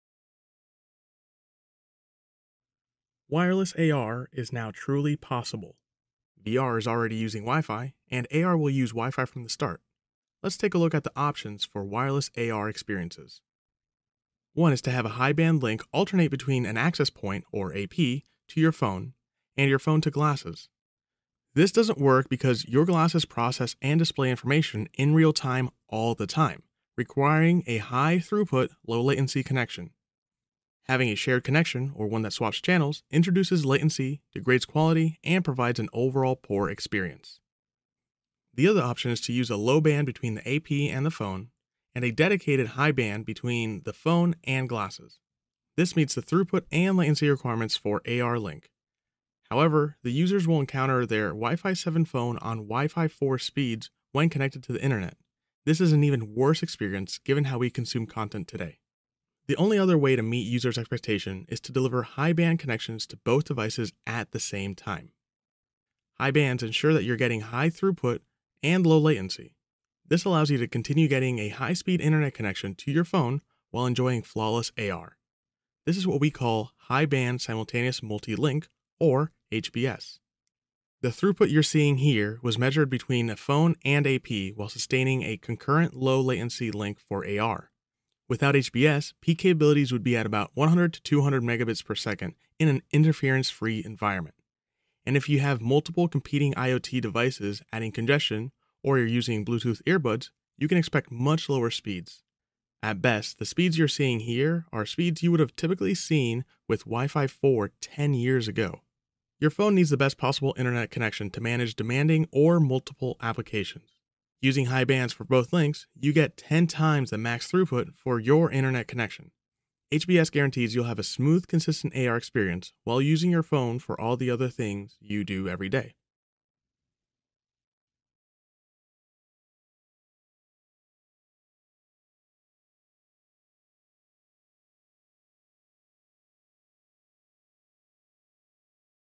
HBS Multi-Link for wireless AR
Wireless AR has two options today, both of which have latency, degraded quality, and an overall poor experience. FastConnect 7800 introduces High Band Simultaneous Multi-Link to deliver two high bands for truly wireless AR.

Wireless AR is a certainty. VR is already using Wi-Fi, and AR will use Wi-Fi from the start. Let’s take a look at the options for the wireless AR experience.

One is to have your high-band link alternate between your access point (AP) to phone and your phone to glasses. This doesn’t work because your glasses process and display information in real time, all the time, requiring a high-throughput, low-latency connection.

Having a shared connection, or one that swaps channels, introduces latency, degrades quality, and provides an overall poor experience.

The second option is to use a low band between the AP and phone and a dedicated high band between the phone and glasses. This meets the throughput and latency requirements for the AR link, but the user’s experience of the internet connection will be that their Wi-Fi 7 phone just became Wi-Fi 4. This is an even worse experience, given how we consume content.

The only way to meet the user’s expectations is to deliver high band connections to both devices at the same time.

High bands at the same time on both channels, called High Band Simultaneous (HBS) Multi-link, ensure you are getting high throughput and low latency. This allows you to continue getting a high-speed internet connection to your phone while enjoying flawless AR.

The throughput you’re seeing here was measured between a phone and AP while sustaining a concurrent low-latency link for AR.

Without HBS, 100-200 Mbps are peak capabilities in an interference-free environment. If you have multiple competing IoT devices adding congestion, or are using Bluetooth earbuds, you can expect much lower speeds. At its best, the speeds you’re seeing here are speeds you’d see with Wi-Fi 4 ten years ago.

Your phone needs the best possible internet connection to manage demanding or multiple applications. Using the high bands for both links, you get 10x the max throughput for your internet connection.

HBS guarantees you’ll have a smooth, consistent AR experience, while still using your phone for all the other things you do every day.